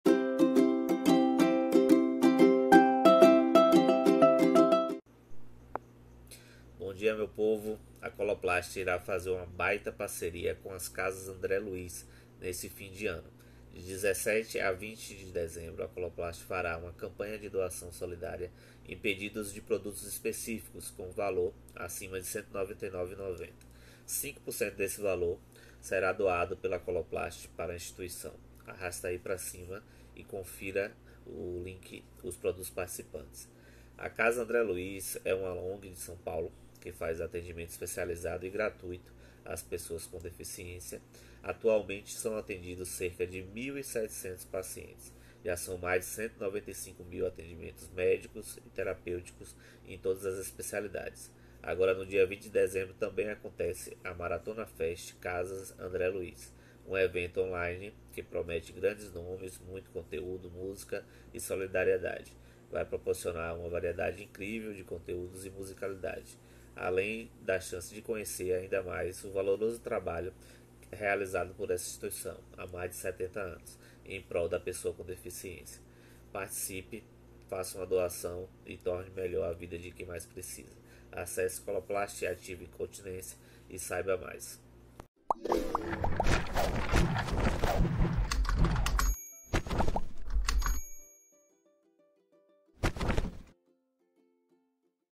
AÇÃO CASAS ANDRÉ LUIZ - COLOPLAST
Bom dia meu povo, a Coloplast irá fazer uma baita parceria com as Casas André Luiz nesse fim de ano
De 17 a 20.12 a Coloplast fará uma campanha de doação solidária
Em pedidos de produtos específicos, com valor acima de R$ 199,90,
 5% do valor do pedido será doado pela Coloplast para a instituição (arrasta pra cima e confira no link os produtos participantes).

(link nos stories)

a @casasandreluizoficial é uma ong de São Paulo que faz "Atendimento especializado e gratuito às pessoas com deficiências." Atualmente, são atendidos cerca de 1.700 pacientes.

Já são mais de 195 mil atendimentos médicos e terapêuticos em todas as especialidades.
agora no dia 20 de dezembro, a Maratona Fest Casas André Luiz (evento online promete grandes nomes, muito conteúdo, música e solidariedade)  vai proporcionar uma variedade incrível de conteúdos e musicalidade, além da chance de conhecer ainda mais o valoroso trabalho realizado pela Instituição, há mais de 70 anos, em prol da pessoa com deficiência.

Participe !
“Faça uma doação e torne melhor a vida de quem mais precisa.”

acesse @coloplastativacontinencia e saiba mais